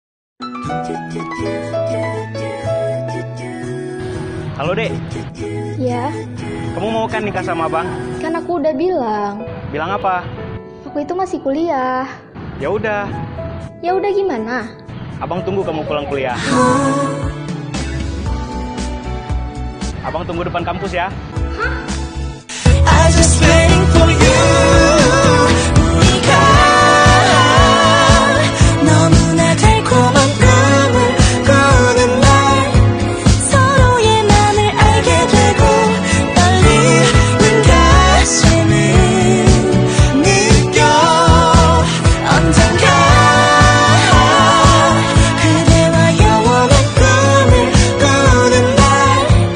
Mahasiswa Unsyiah ngajak nikah di perpus
Ketangkap mhs unsyiahbdi perpus ngajak nikah